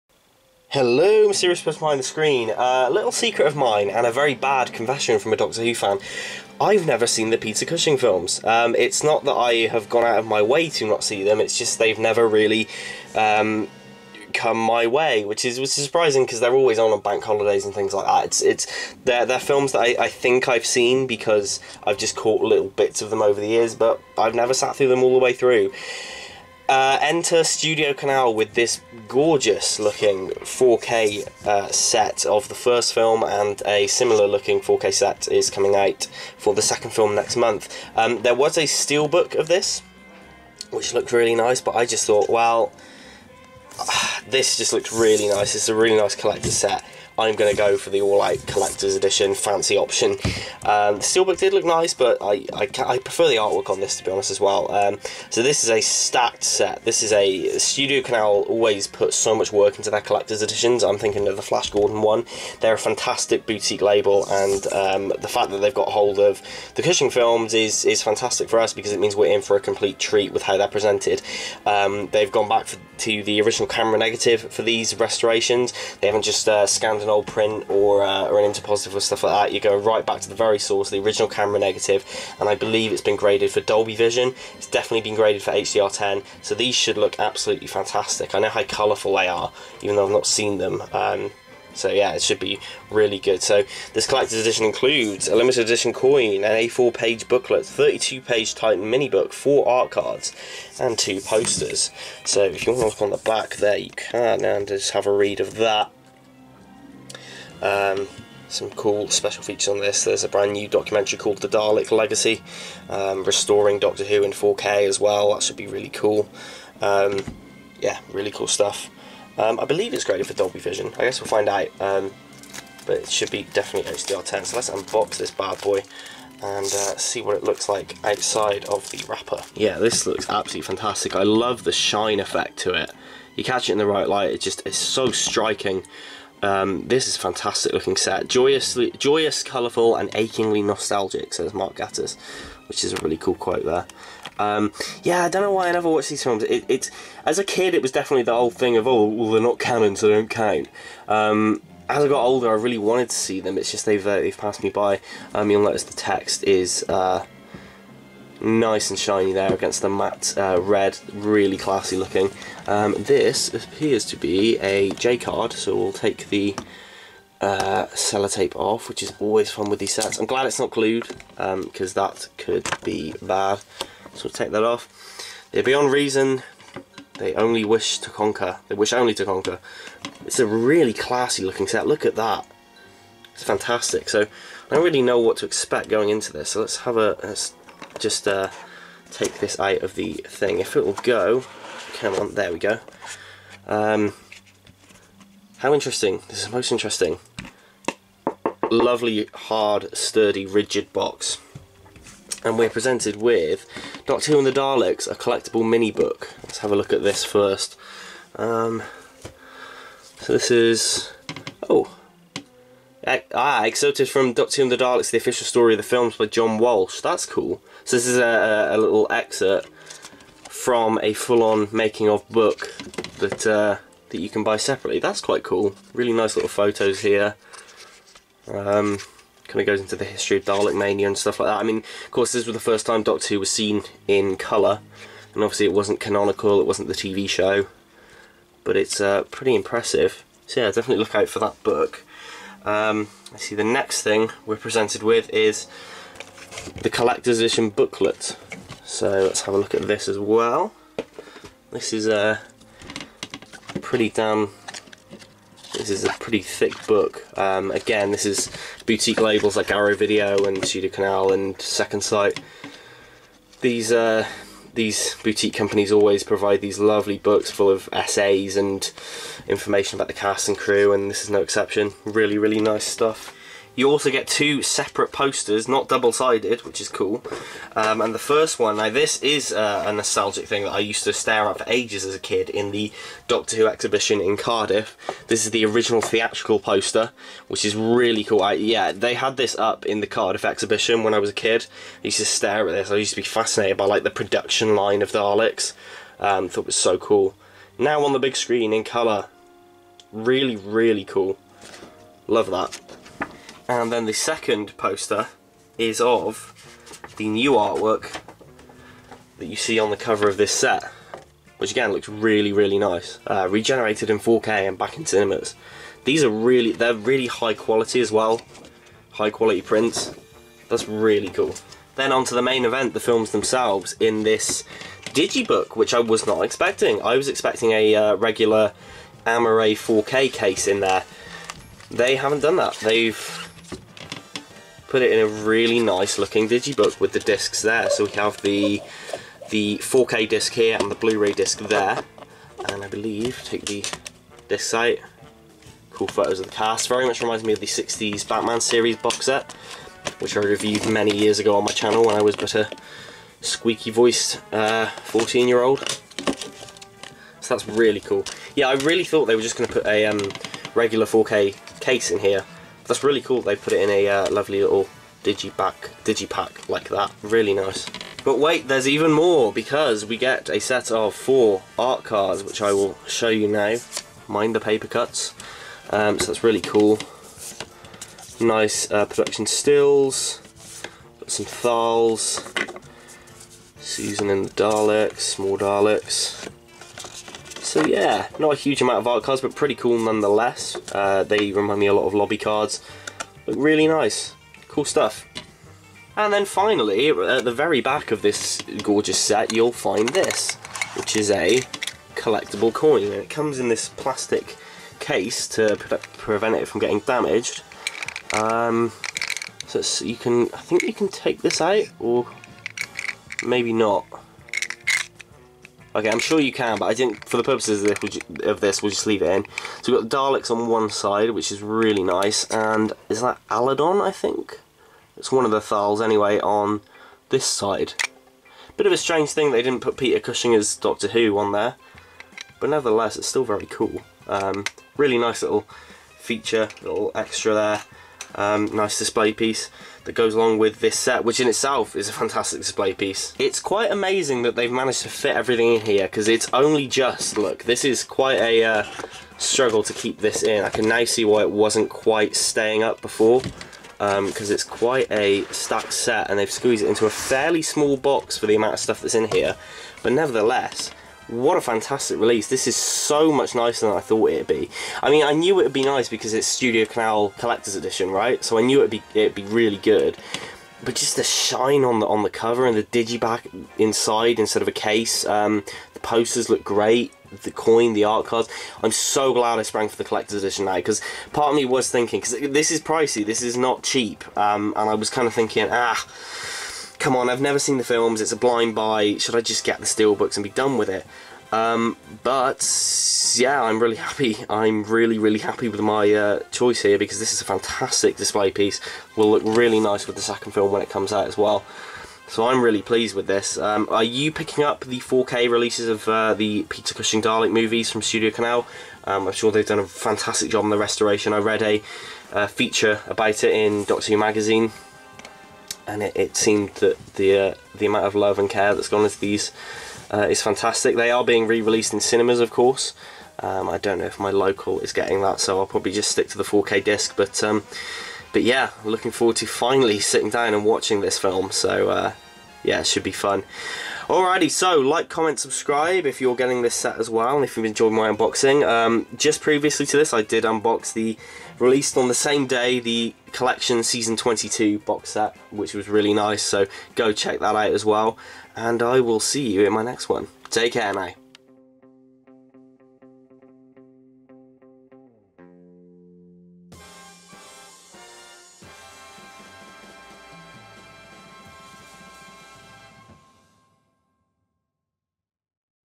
Dr. Who and the Daleks 4K Collector's Set Unboxing!
Dr Who and the Daleks has come to 4K with an absolutely gorgeous premium package...